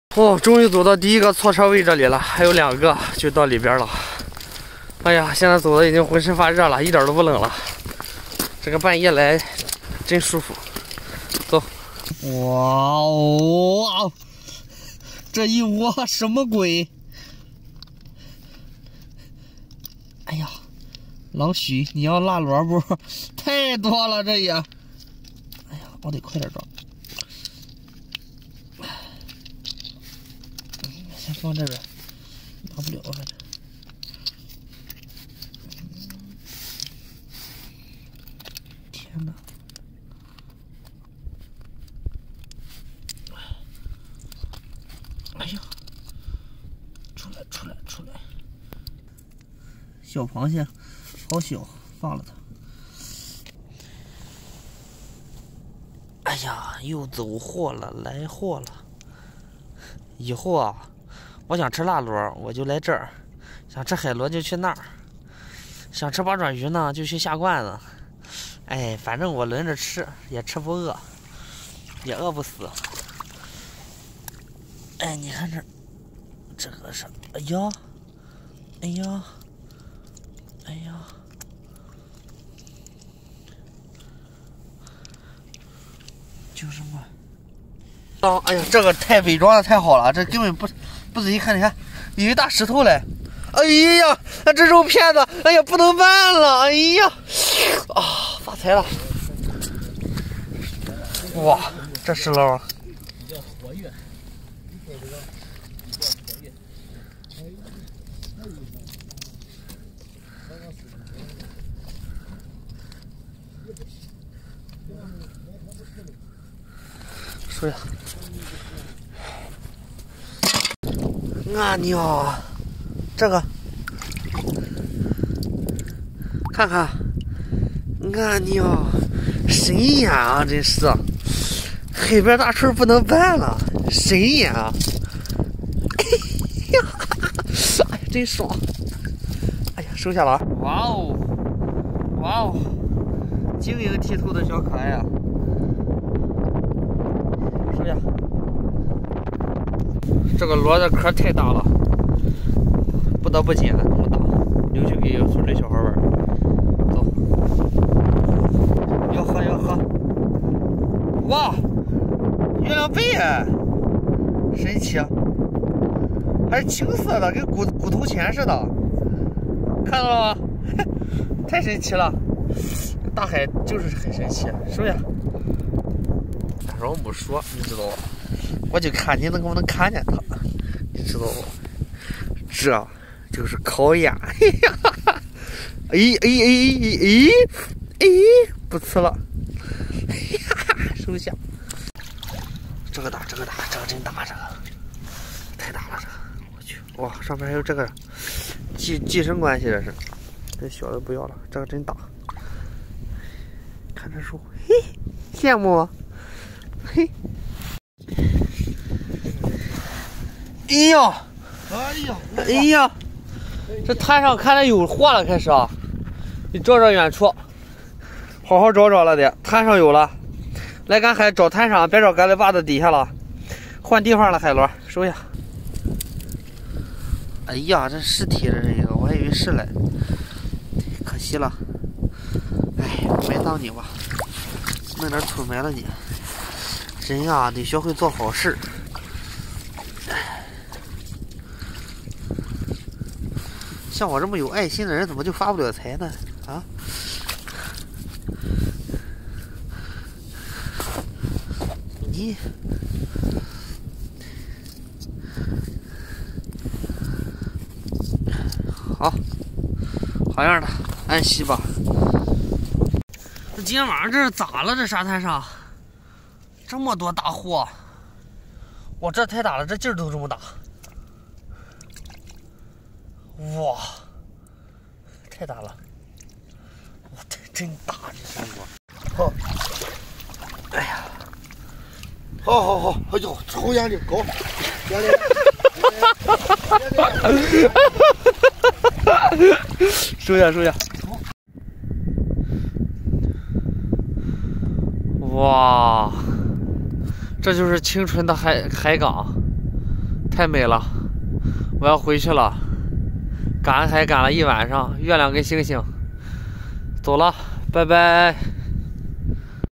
通宵赶海，沙滩上都是亮晶晶的小海萝卜花，二蛋走两步就能遇一个Erdan went to the sea and found sea radish on the beach【渤海二蛋】
大家好，这里是渤海二蛋，带大家一起赶海、抓海鲜咯，